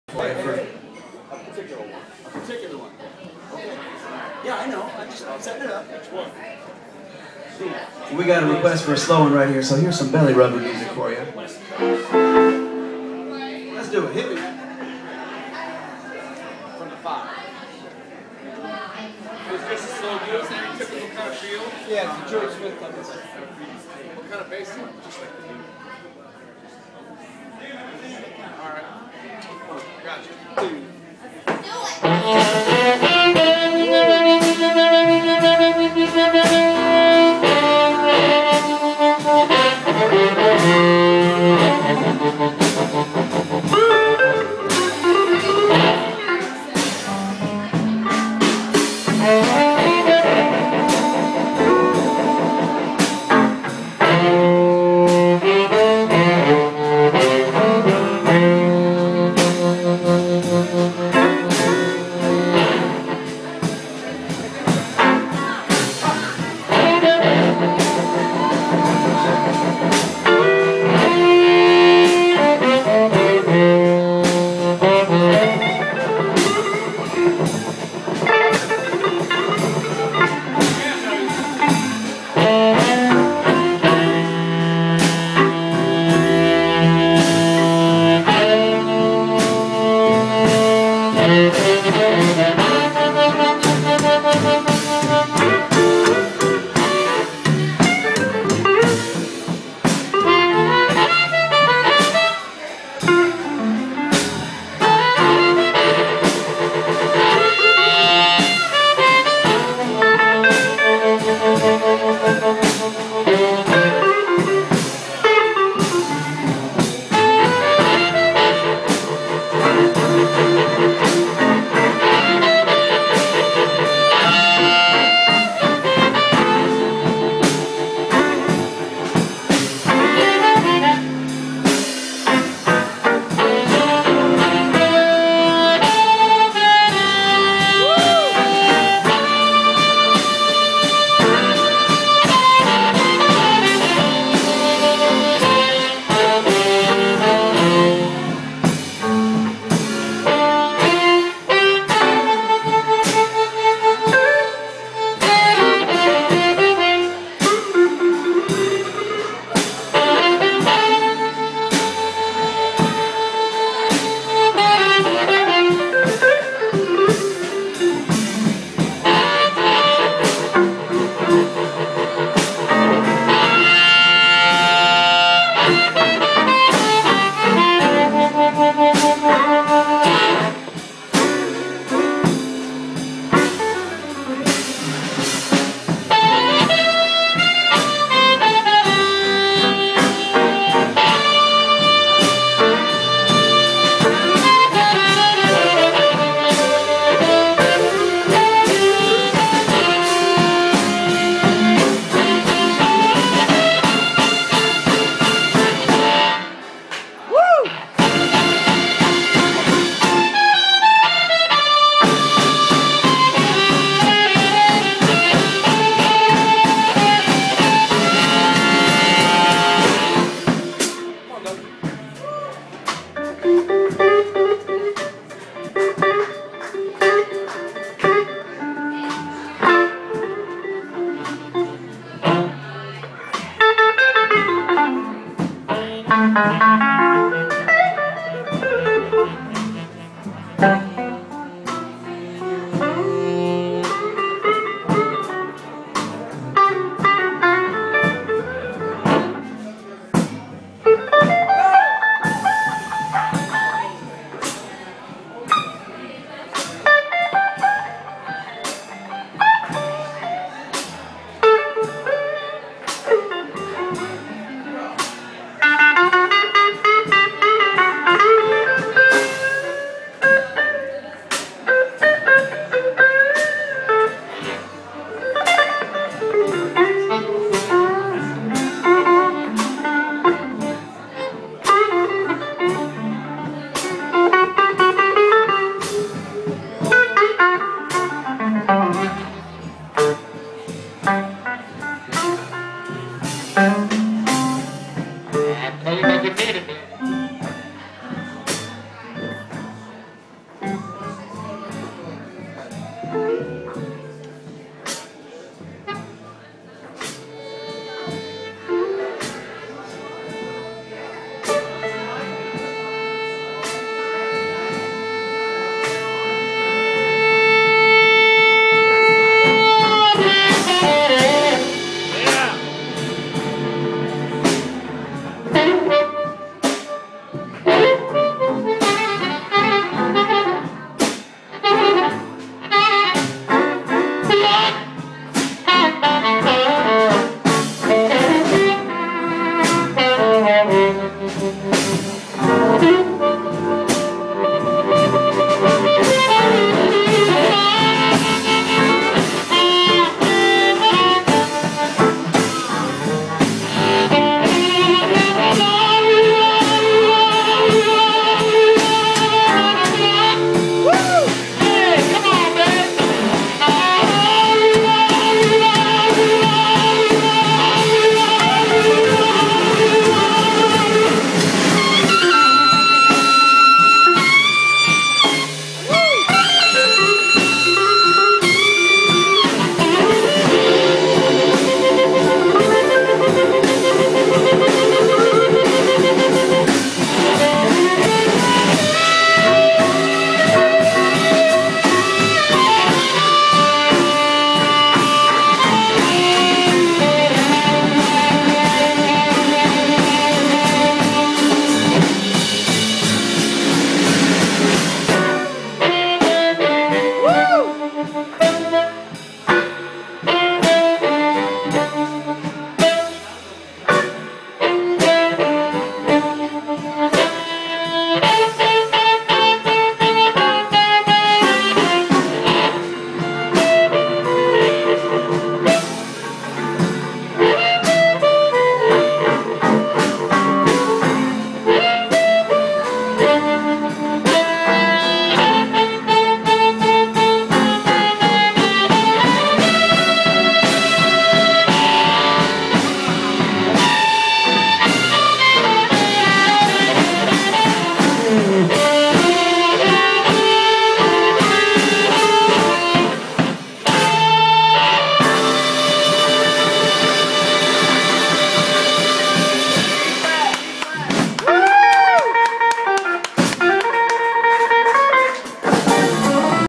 Dennis Gruenling at Lobsta's Anna Maria Island
At Lobsta's in Anna Maria Island Florida, Dennis playing with Doung Deming and The Jewel Tones. Great gig, very nice venue, good food!!